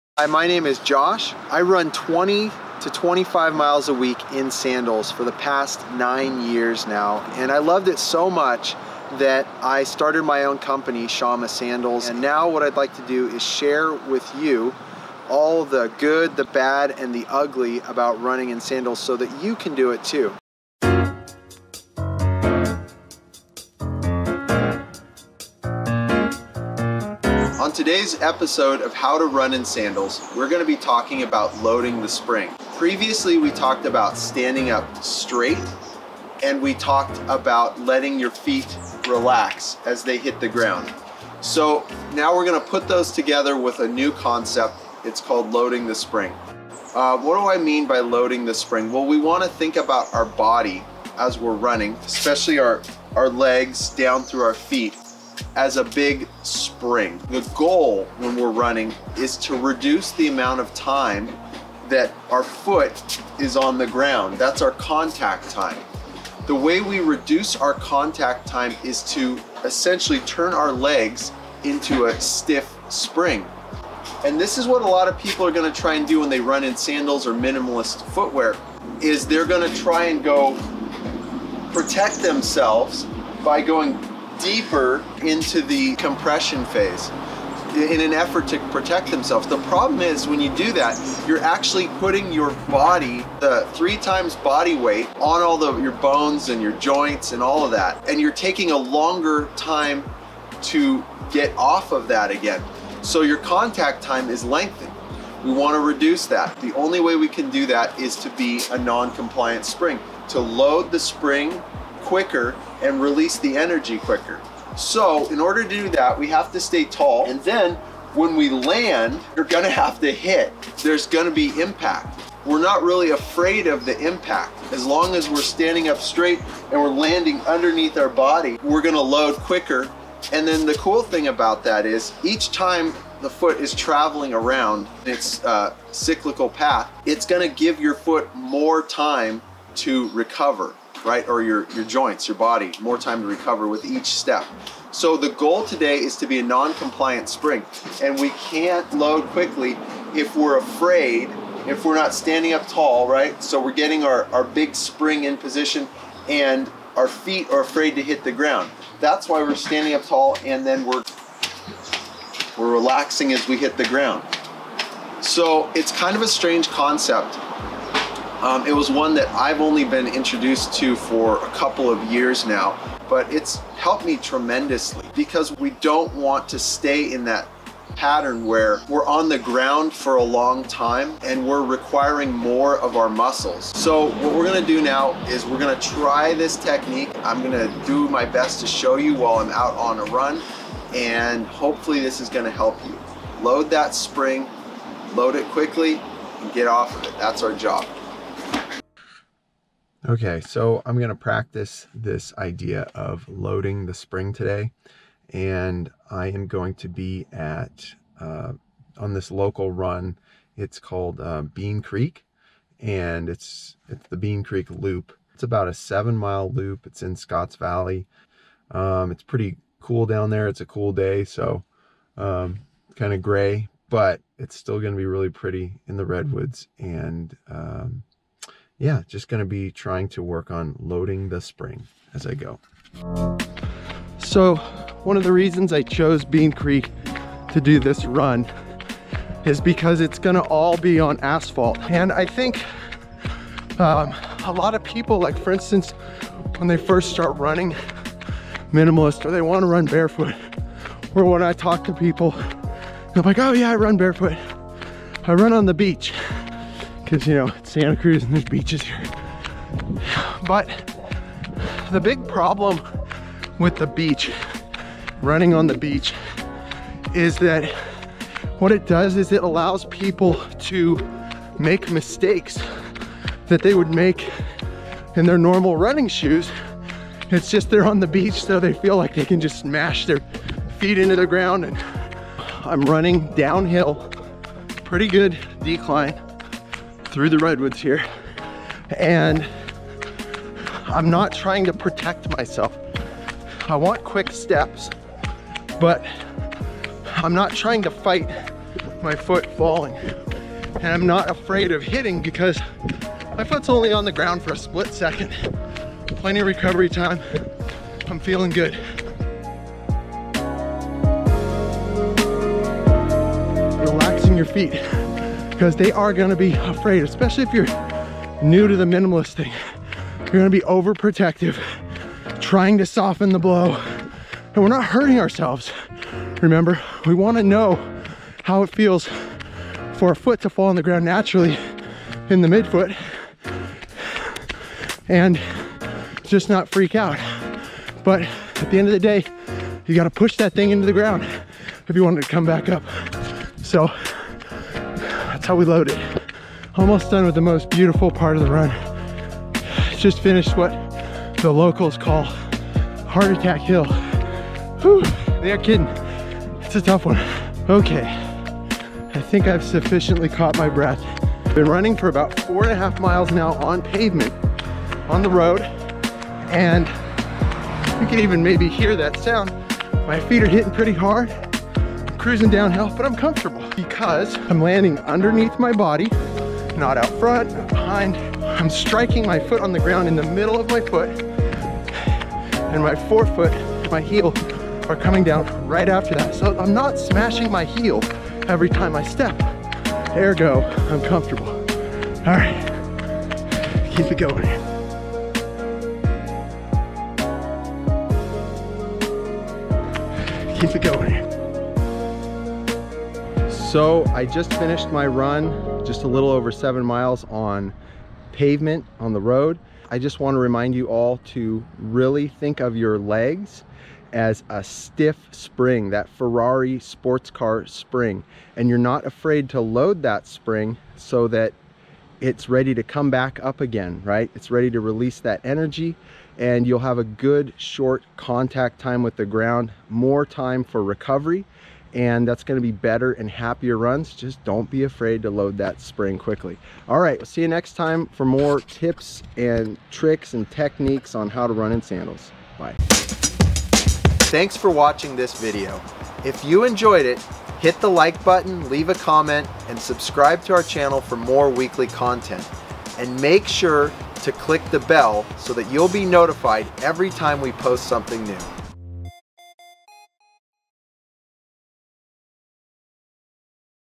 HOW TO RUN IN SANDALS : Episode 3 "Load the Spring"
How to run in sandals, a new series from Josh and the team here at Shamma Sandals. Not sure if you want to make the switch to sandals or minimalist shoes? Want to work on your running technique? Come along  and explore the terrain with Josh as he explains his running technique and hit the trails in his sandals.
Want to know what's new? Sign up to get your free Barefoot Transitioning Guide, created with Mike Prevost.